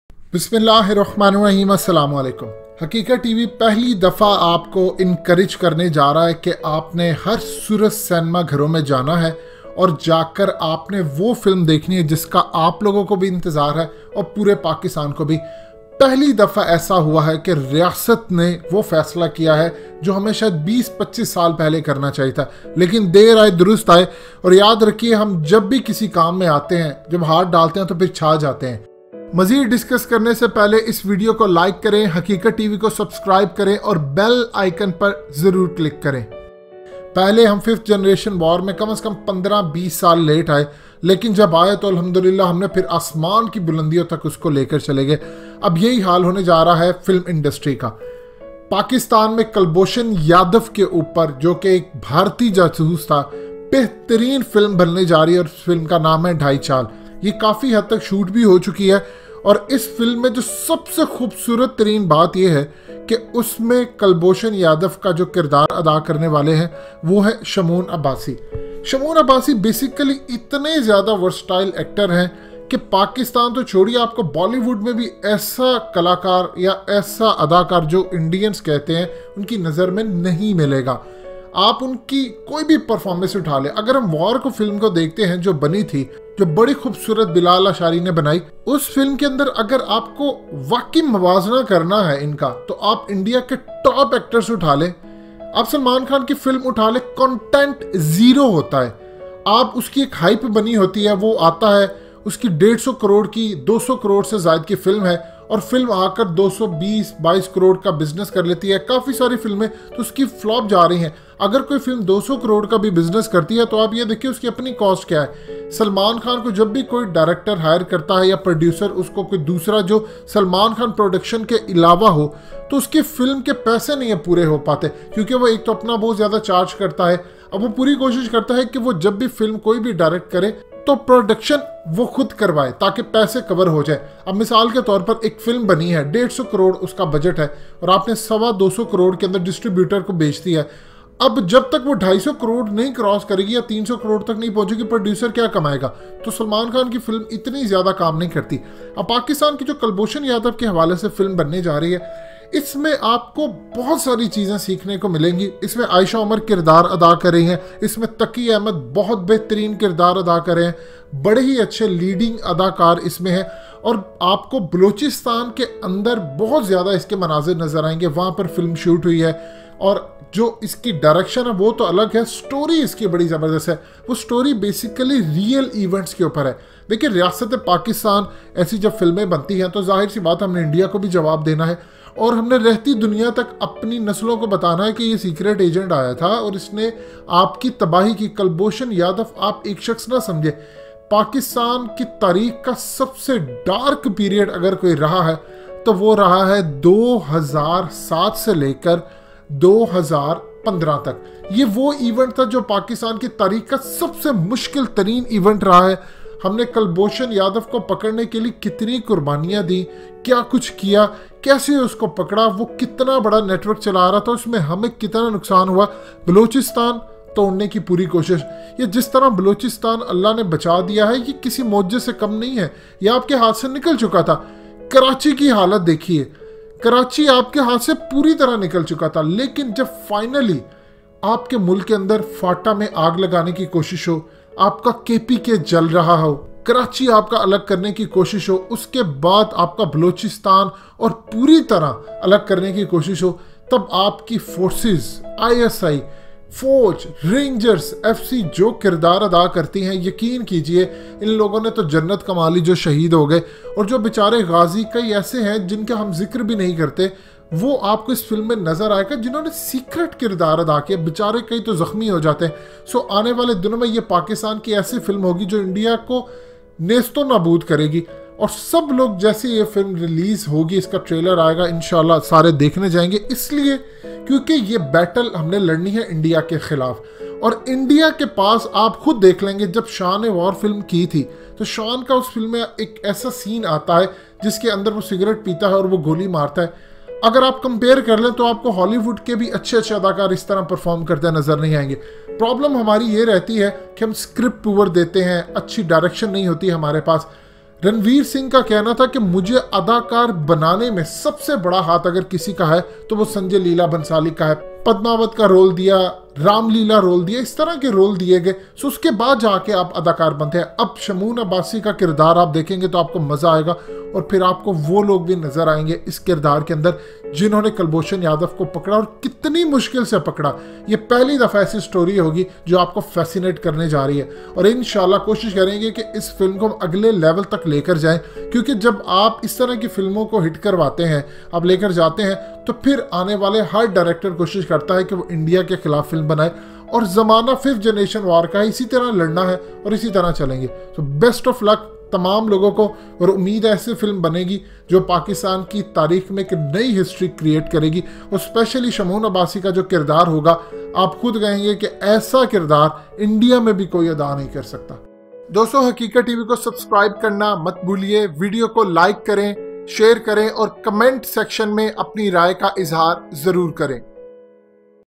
An Upcoming Pakistani Movie Dhai Chaal is Ready to Hit Box Office
Dhai Chaal is an upcoming Pakistani movie based on true events and it may be one of the most entertaining movie in the history because the subject is quite amazing in this way.